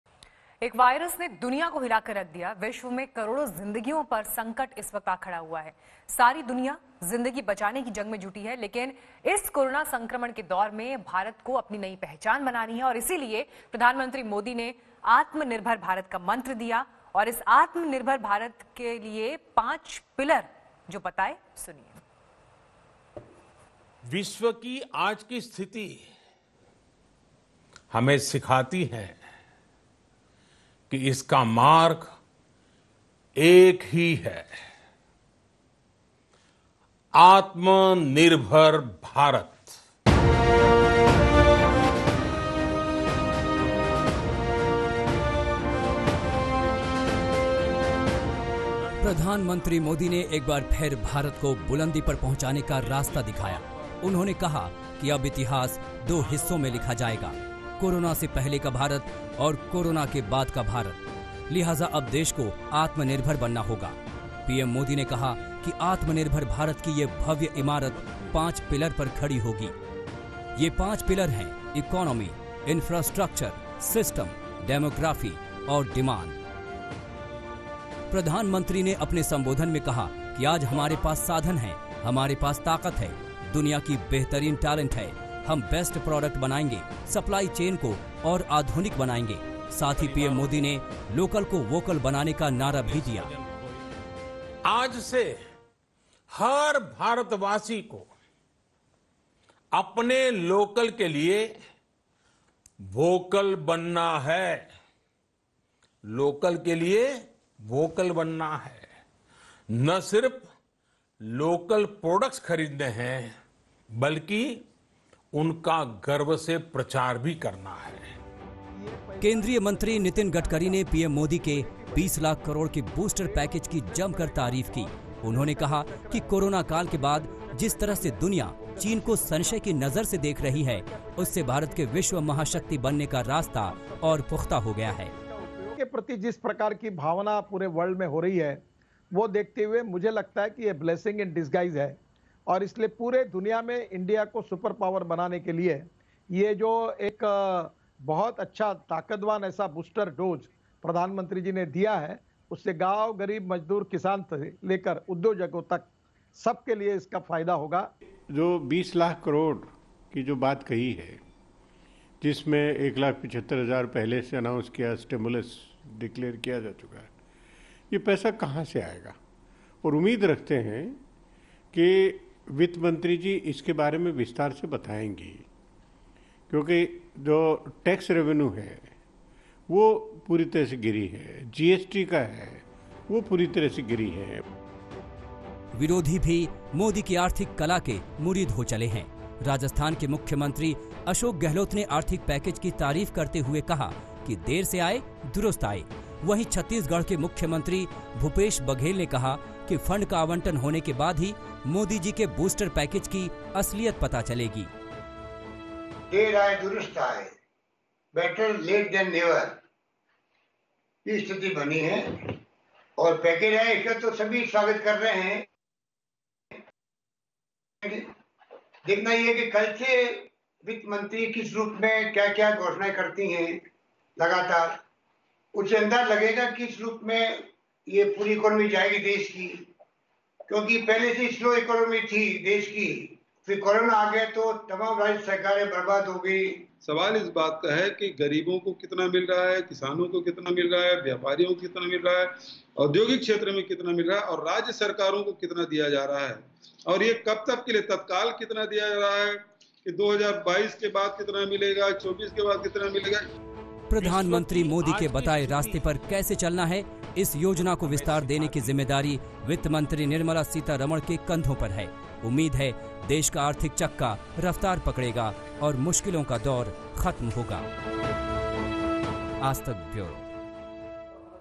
Modi ने बताए 'आत्मनिर्भर' भारत के 5 पिलर, बोले 'हमें बचना भी है आगे बढ़ना भी है'
एक वायरस ने दुनिया को हिलाकर रख दिया है, विश्व में करोड़ों ज़िंदगियां को संकट का सामना कर रही है। सारी दुनिया जिंदगी बचाने में जंग में जुटी हुई है। लेकिन इस कोरोना काल में भारत को अपनी नई पहचान बनानी है। और इसीलिए प्रधानमंत्री मोदी ने
आज आत्मनिर्भर भारत का मंत्र दिया है। और इस आत्मनिर्भर भारत मंत्र के 5 पिलर भी बताए हैं।